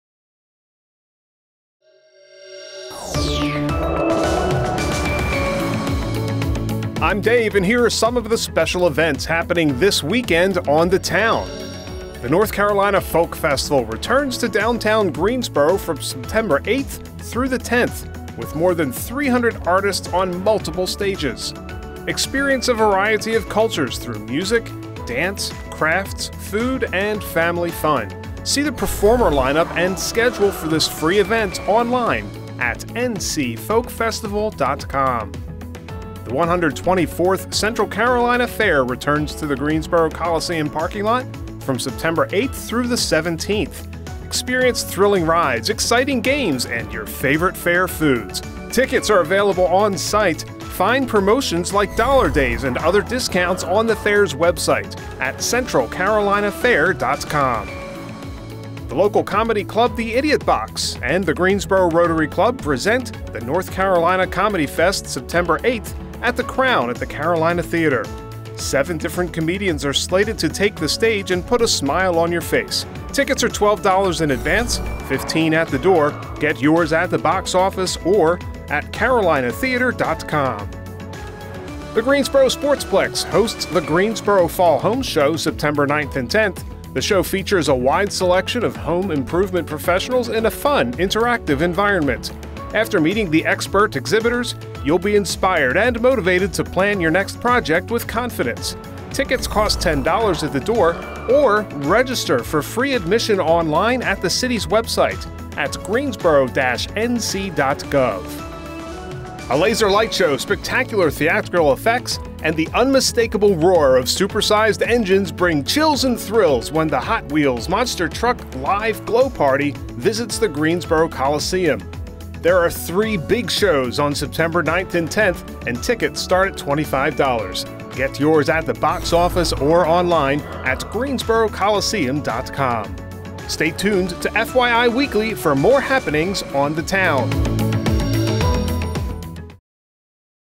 On the Town - Week of September 4, 2023
"On the Town" is a quick video segment from FYI Weekly that tells residents of Greensboro about things happening around the City.